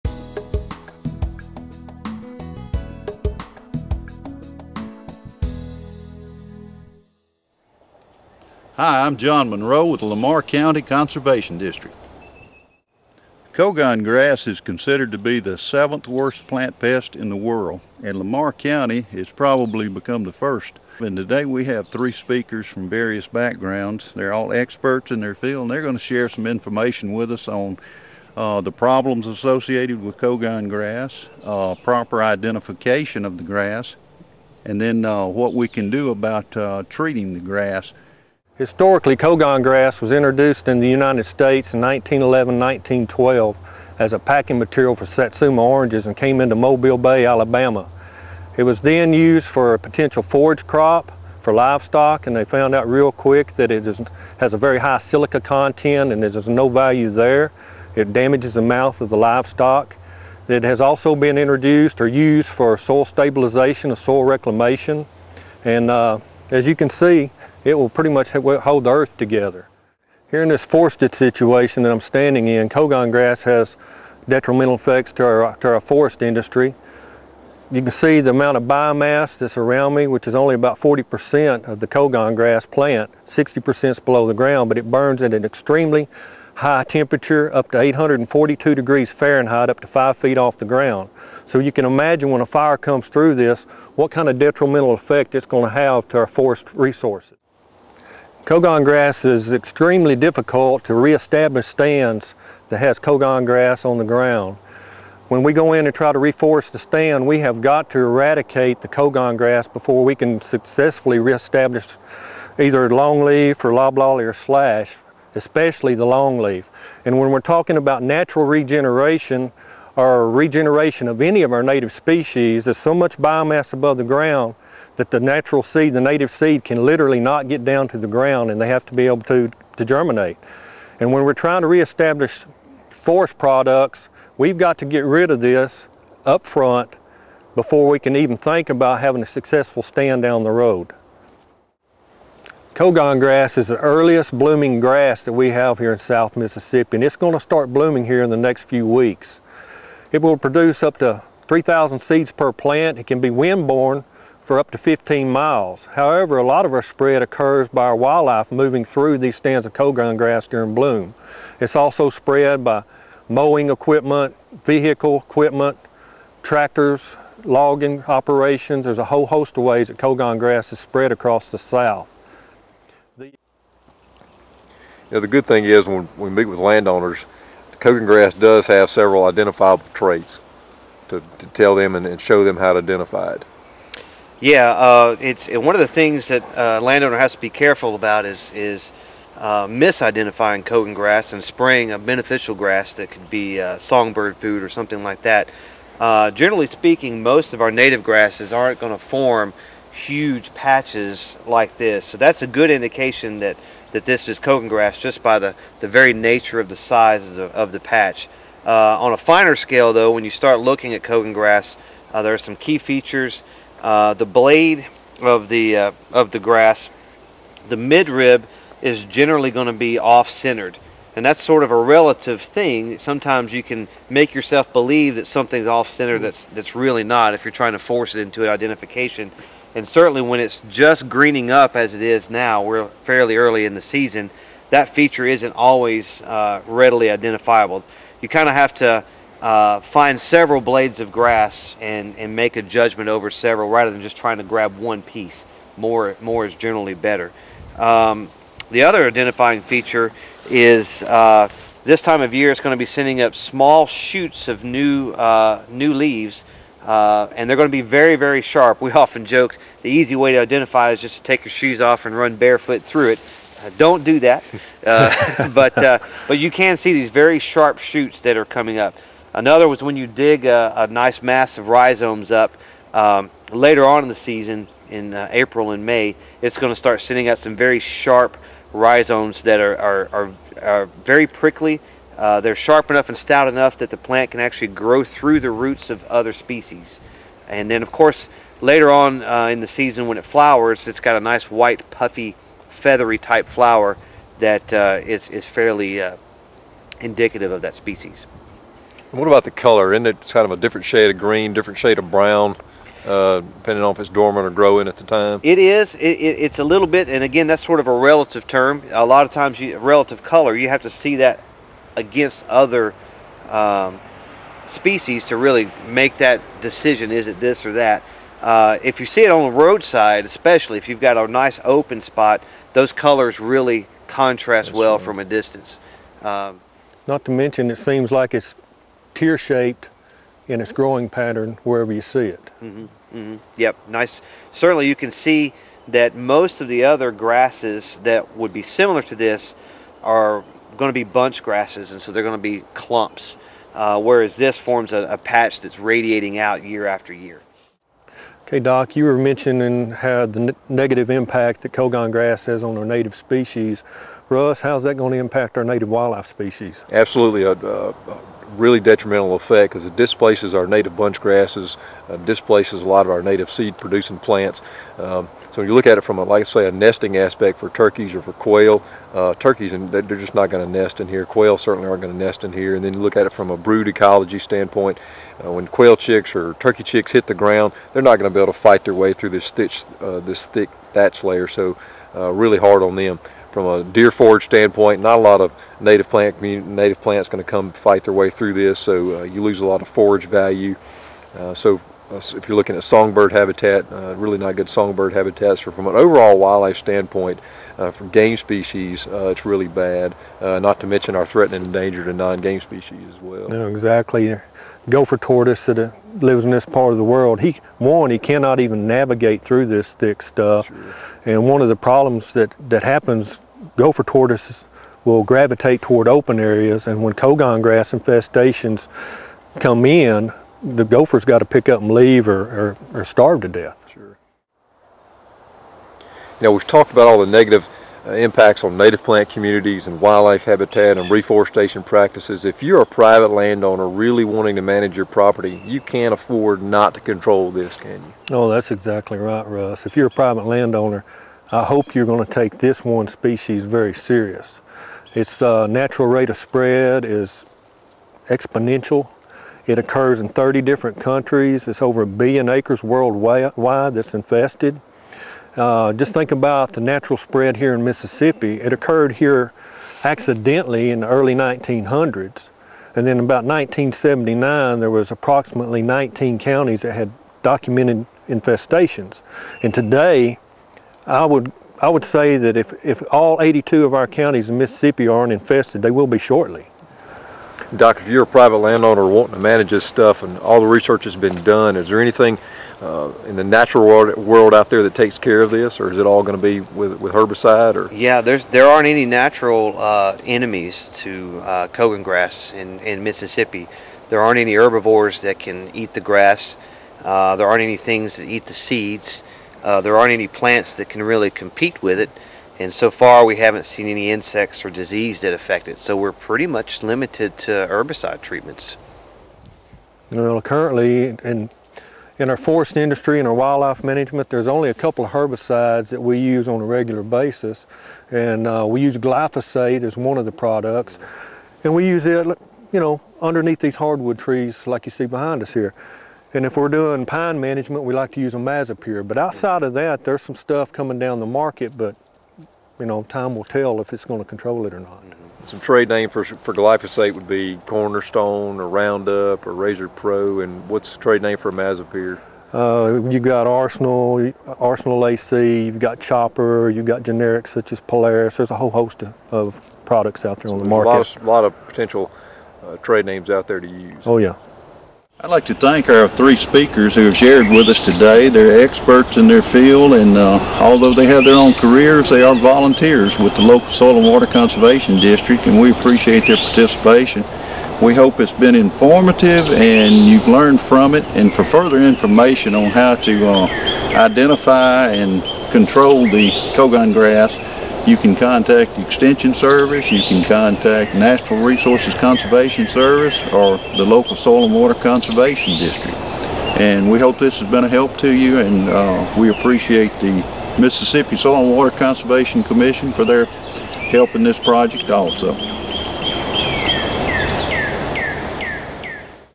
Cogongrass,The Perfect Weed
History of Cogongrass and how to identify it.